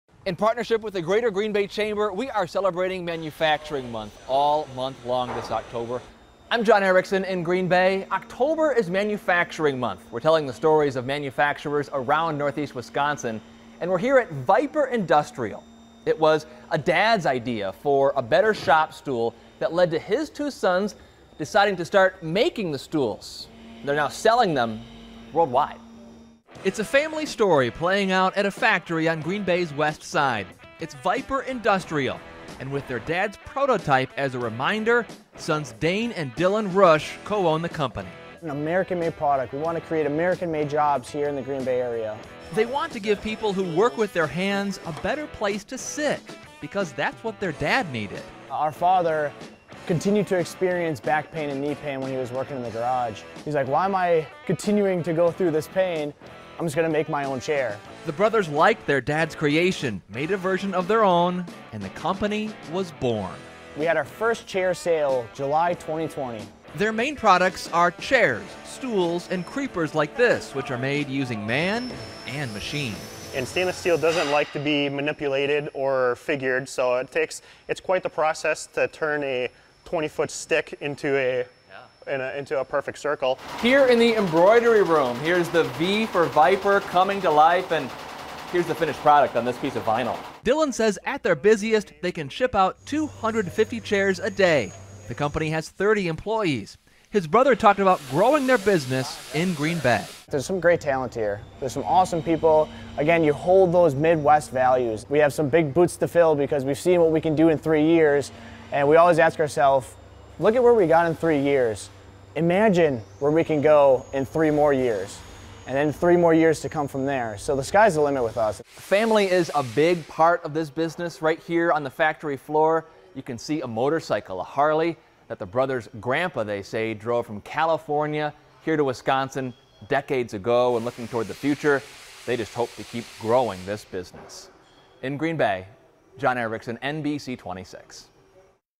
Father's idea inspires brothers' company: Vyper Industrial
Vyper Industrial's central products are shop stools, chairs, and creepers (enabling someone to lie flat and roll under a vehicle).